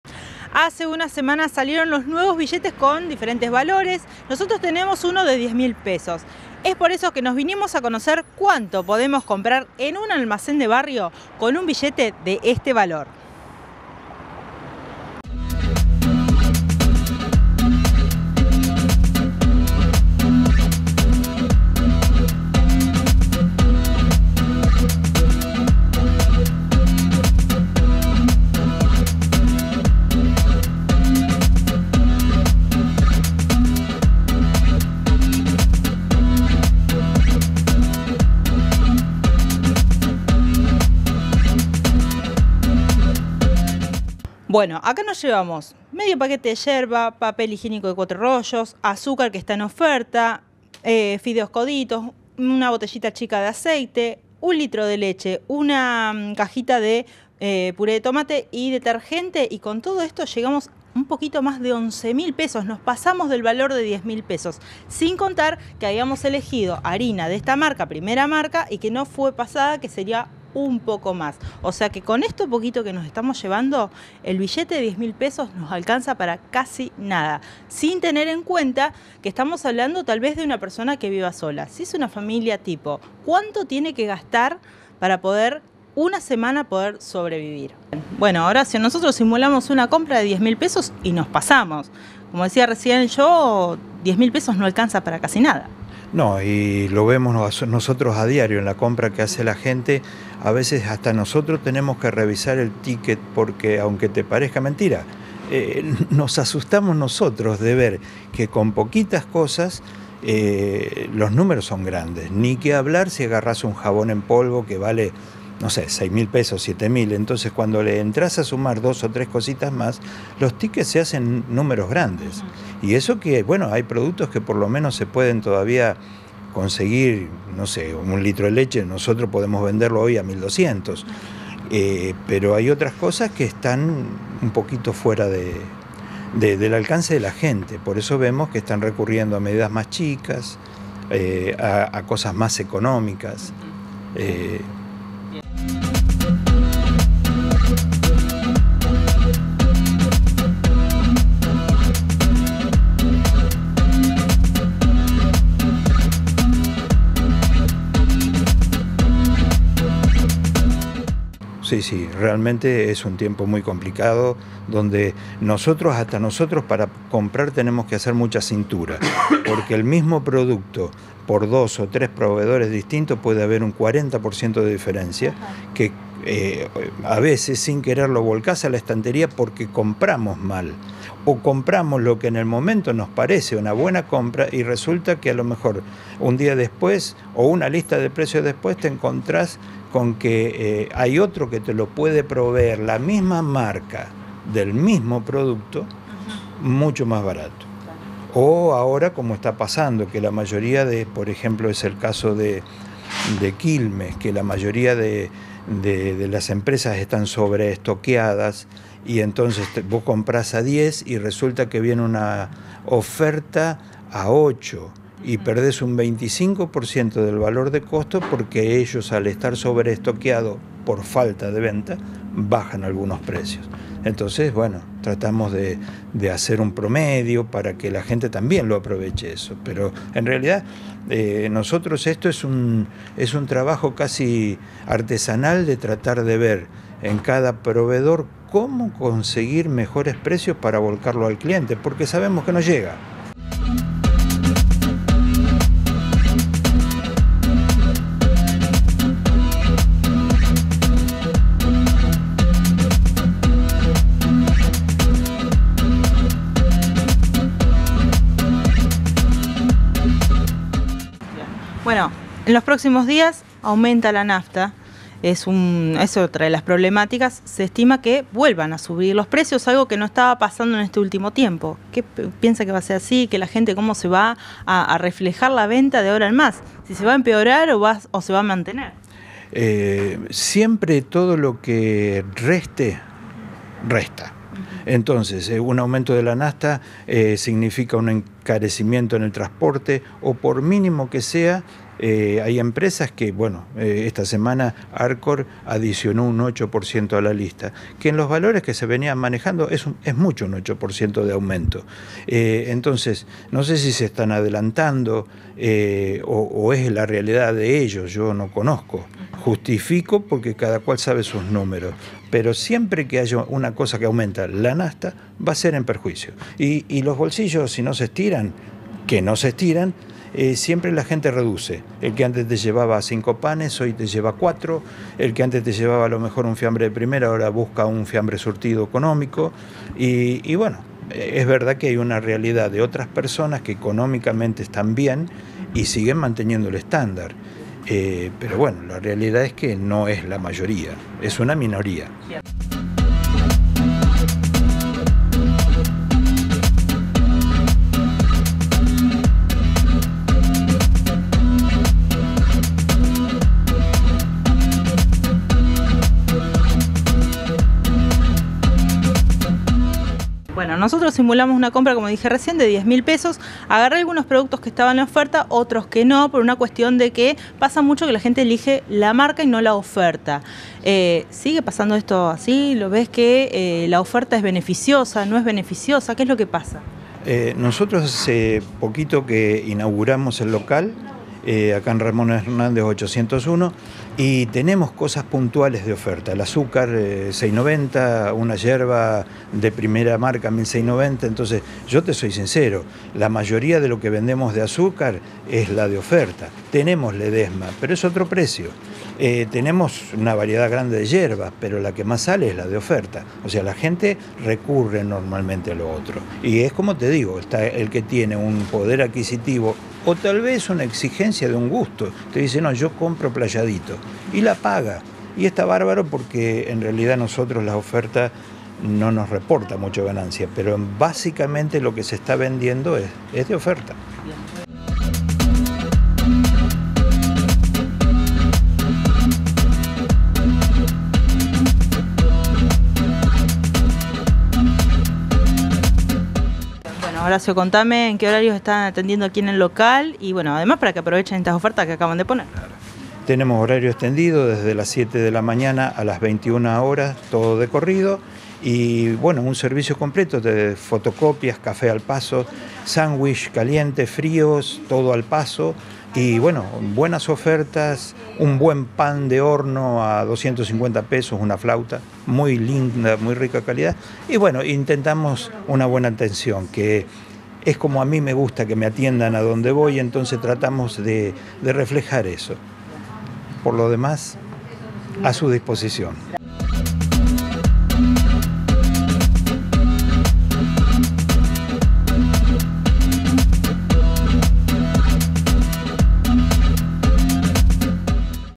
ALIMENTOS BÁSICOS: ¿Qué compramos con el billete de 10 mil pesos?
10N en la calle

Comunicate con 10Noticias 👉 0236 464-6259